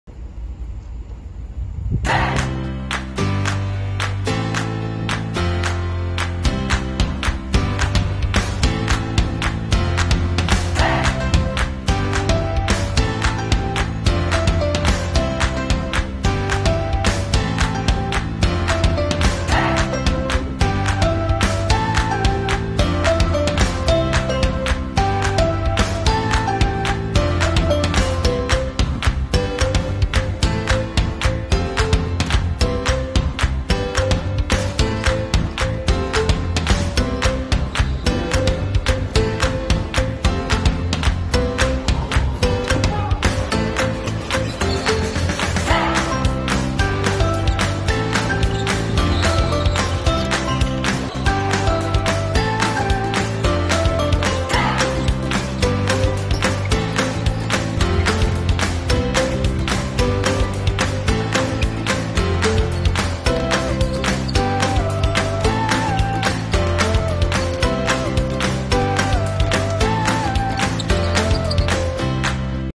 Life at SJTU, Let's play badminton!
It is an excellent time to work out! How about playing badminton at the sports center on the SJTU Minhang campus?